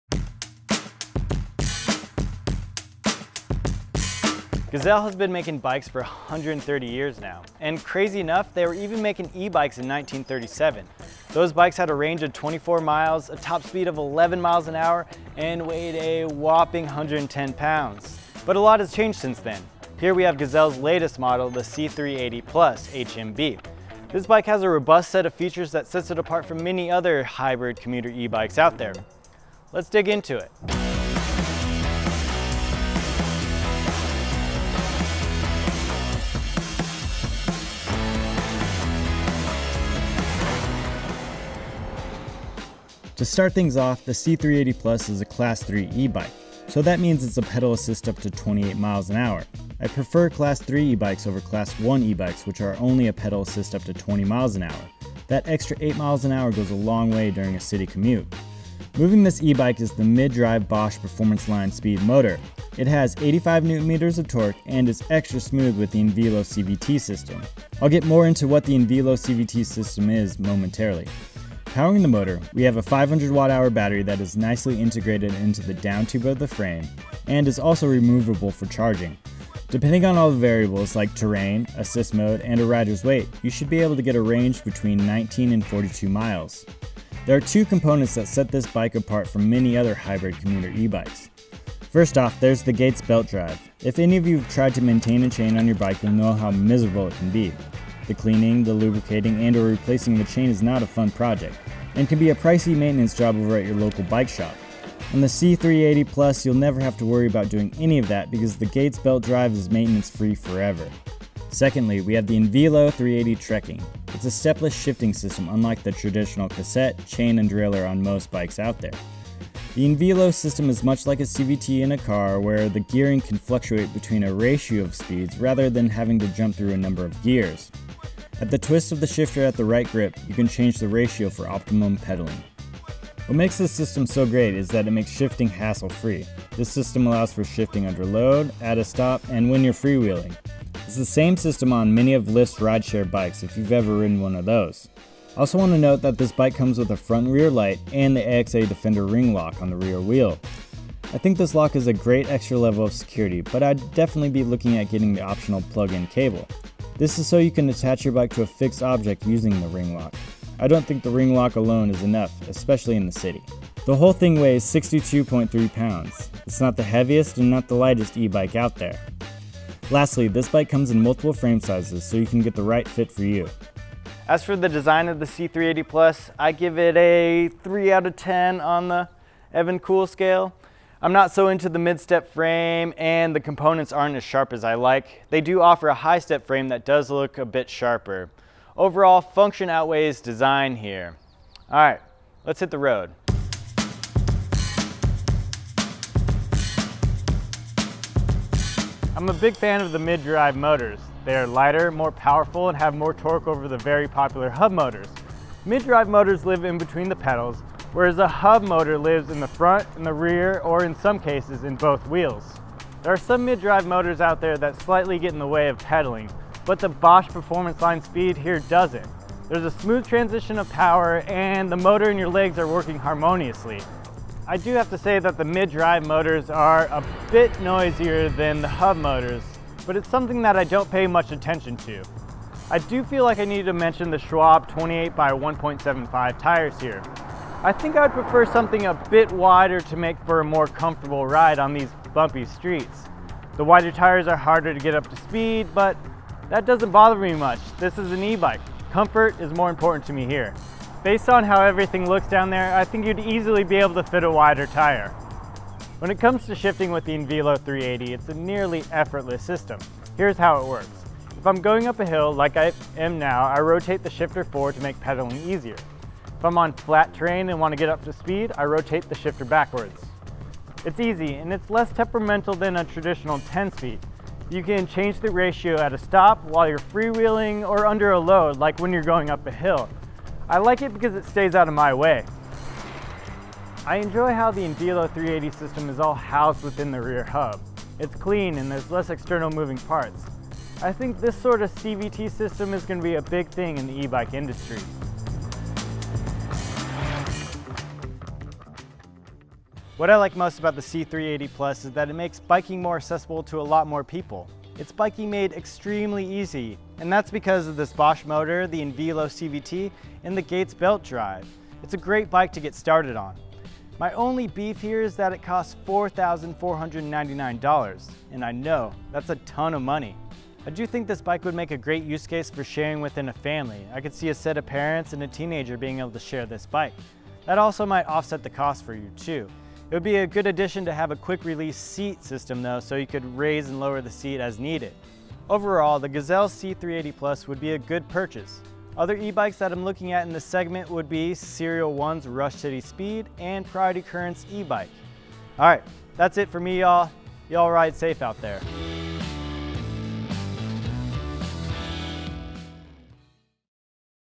This e-bike can make your commute EASY
The C380+ HMB (Hybrid Mid-Drive Bosch) has a robust set of features that set it apart from the many other Hybrid/Commuter e-bikes out there.

Written & Presented by Evan Lee Miller
Produced by Evan Lee Miller